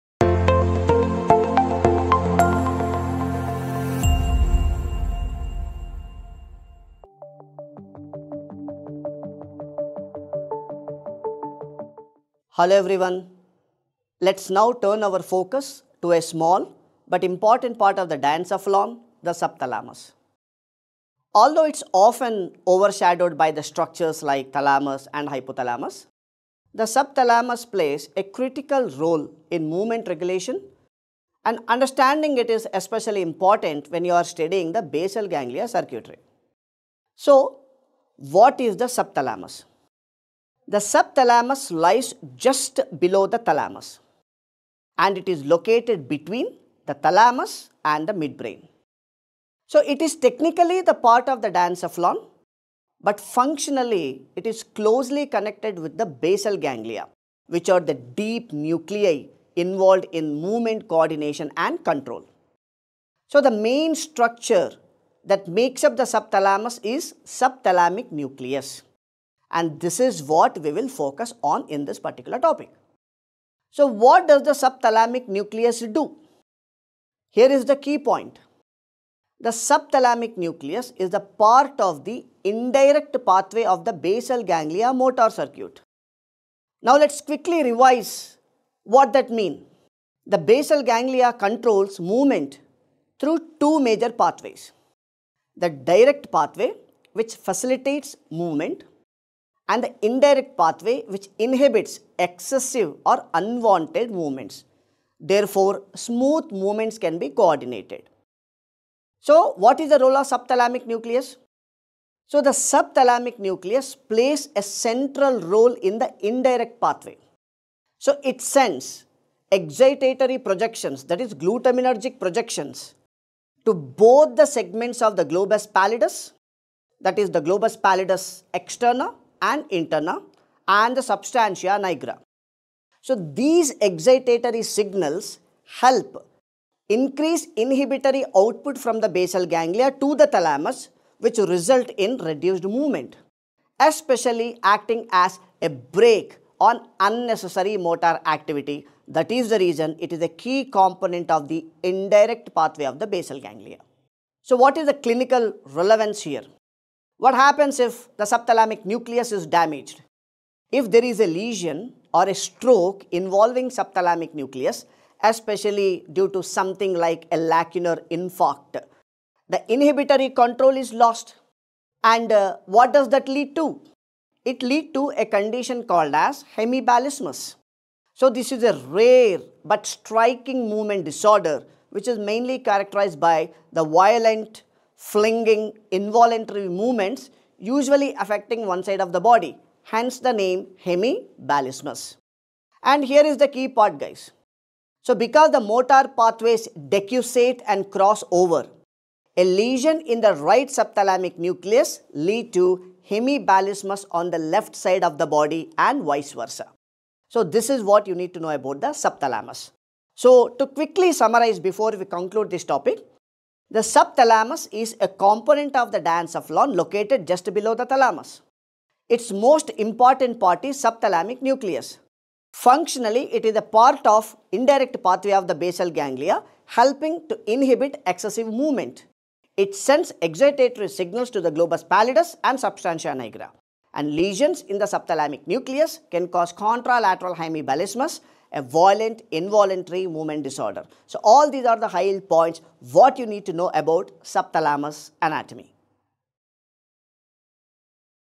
Subthalamus Anatomy 🧠 Structure, Function & Clinical Relevance | Basal Ganglia Integration
Subthalamus 🧠  Structure, Function & Clinical Relevance | USMLE Step 1 | Basal Ganglia Integration

In this high-yield neuroanatomy video for USMLE Step 1, we explore the often-overlooked but incredibly important Subthalamus, with a deep focus on its anatomy, function, and clinical relevance—especially within the basal ganglia motor circuitry. Located just beneath the thalamus, the subthalamus is part of the diencephalon, and its core component—the subthalamic nucleus—plays a pivotal role in movement regulation through the indirect pathway of the basal ganglia. 🎯

The subthalamic nucleus is functionally connected to key movement centers, including the globus pallidus (GPe and GPi) and the substantia nigra, and it sends excitatory glutamatergic inputs that ultimately lead to inhibition of thalamic output, thereby suppressing excess motor activity. In simple terms, the subthalamic nucleus acts like a brake to prevent unwanted or excessive movements. ⚙️🧠

Clinically, this structure becomes extremely high yield when damaged—especially in lacunar strokes—which can disrupt the indirect pathway, leading to a rare yet dramatic condition called hemiballismus. Patients with this disorder exhibit violent, involuntary flinging movements of the contralateral limbs, due to disinhibition of motor output. 🚨 Because motor control is contralateral, a right subthalamic lesion causes left-sided hemiballismus, and vice versa.

By the end of this video, you’ll confidently understand:
– Where the subthalamus is located
– How the subthalamic nucleus fits into the indirect pathway
– Why it sends excitatory signals to basal ganglia structures
– What happens when it’s damaged
– And how to apply this knowledge to Step 1 neuroanatomy vignettes

This is a must-know structure for mastering movement disorders and for scoring high on basal ganglia-related questions in neuroscience and internal medicine modules.